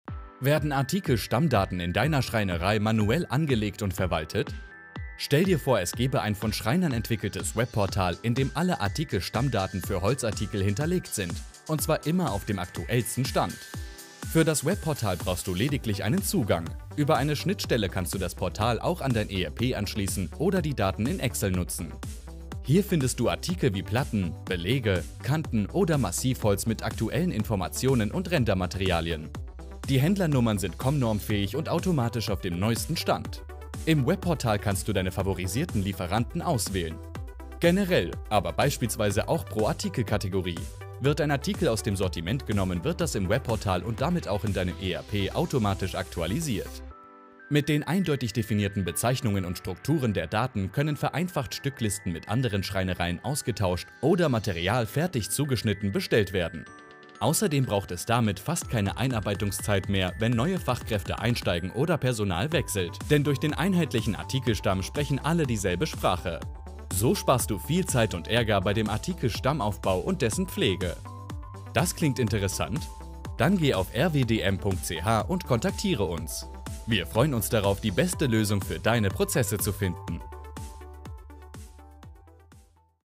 Werkstoff-Datenstamm rwdm
Der neue Werkstoff-Datenstamm für alle Schreinereien bleibt ohne Pflege immer auf dem aktuellsten Stand. Und er ist ComNorm-fähig.